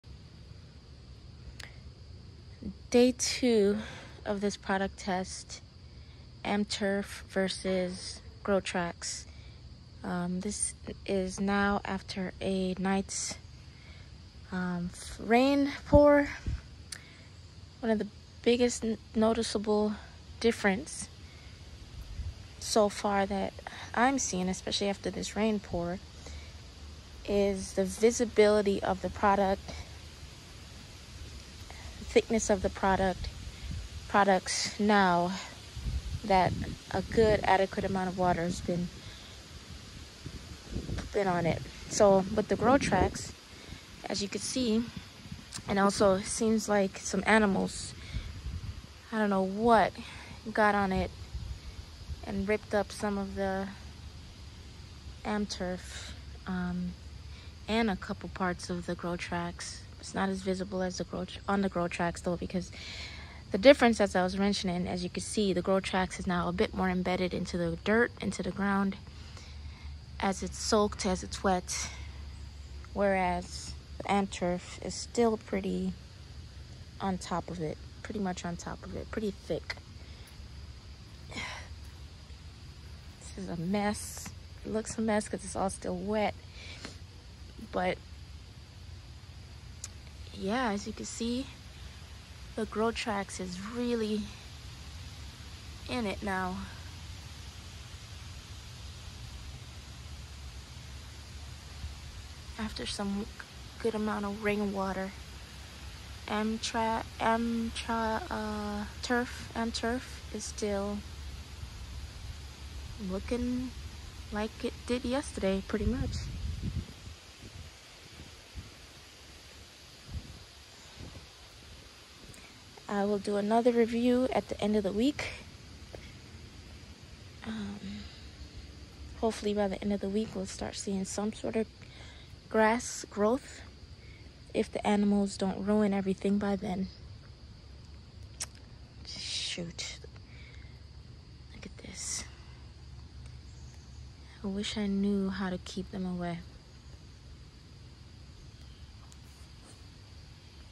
Grotrax Vs Amturf Day 2!
After a big rain storm last night, both products are now fully wet with an adequate amount of water. Let’s see the difference thus far!